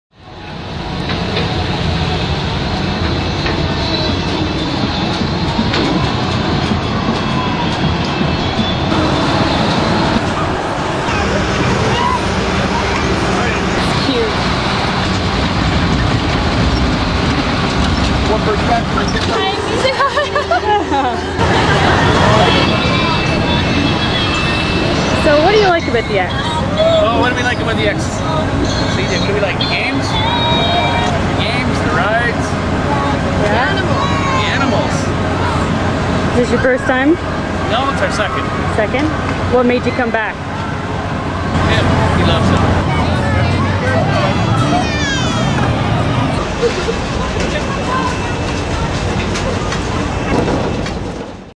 A day at the Peterborough Exhibition
Reporter Lindsey Cole gets a peek at the Peterborough Exhibition.